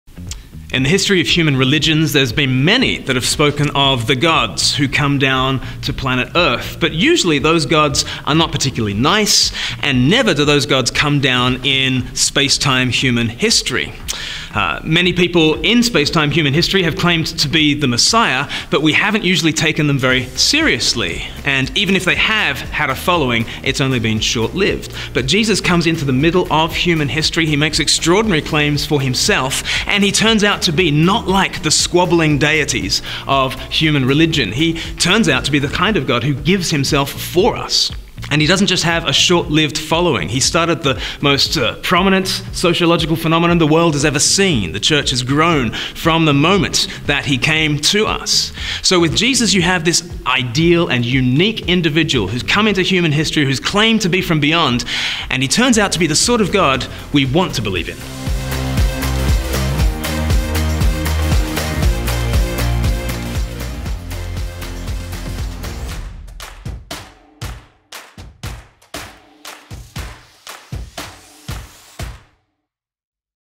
How Is Jesus Different from Any Other Messiah?
Glen Scrivener explains how Jesus Christ is the sort of God we want to believe.